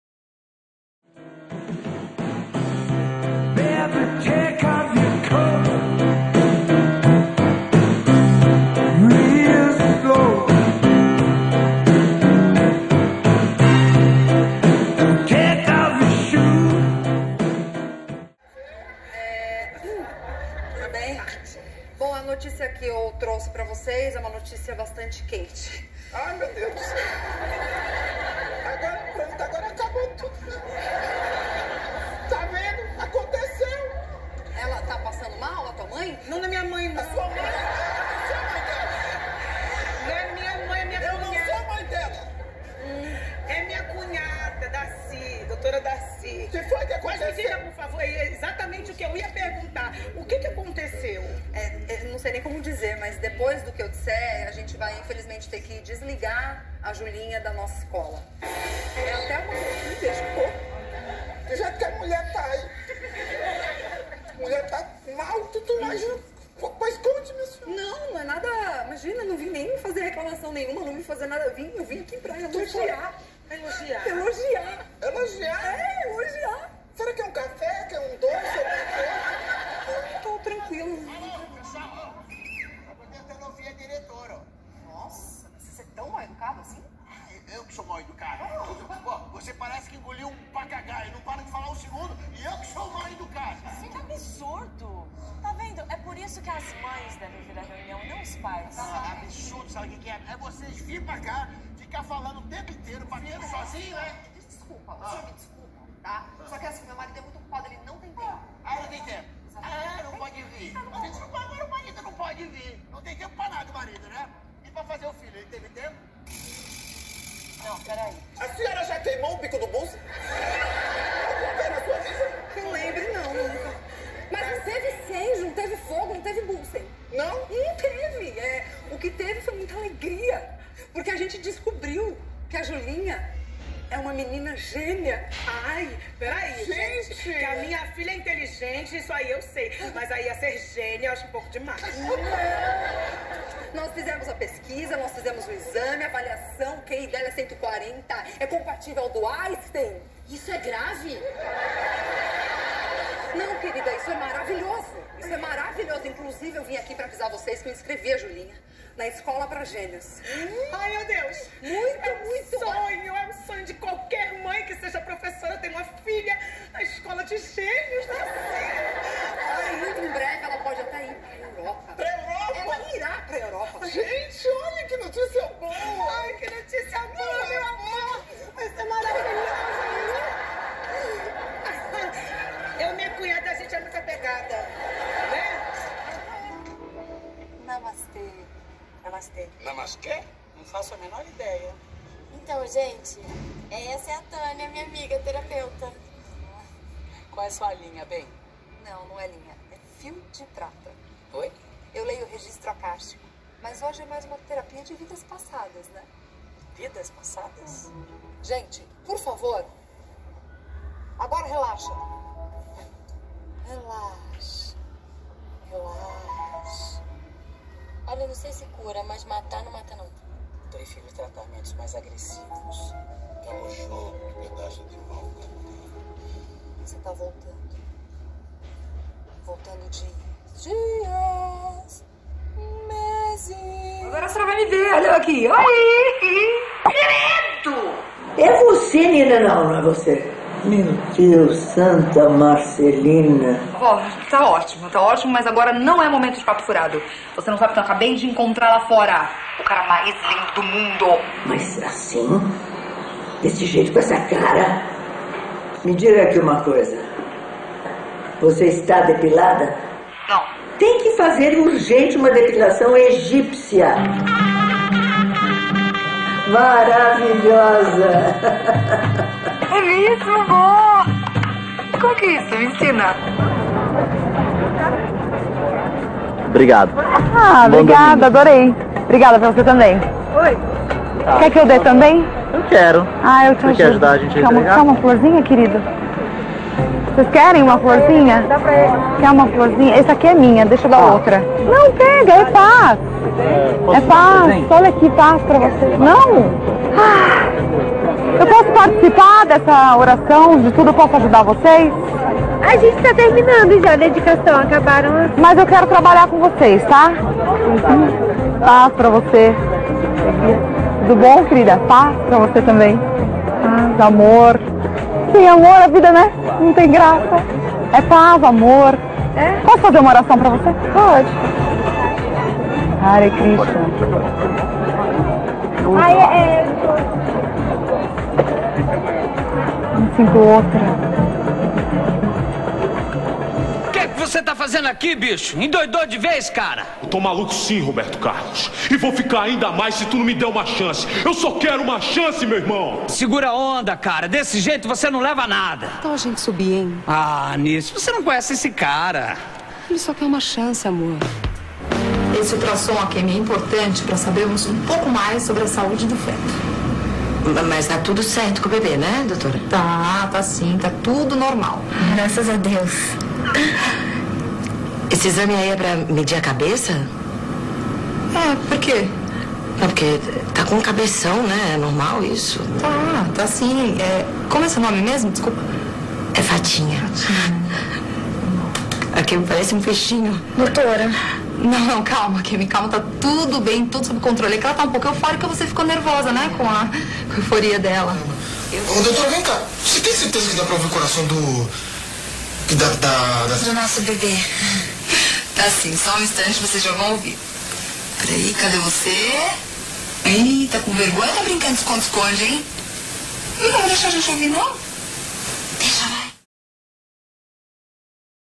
Demo Reel Wanessa Morgado
Apanhado de trabalhos realizados em séries, seriados e novelas.____________________________________________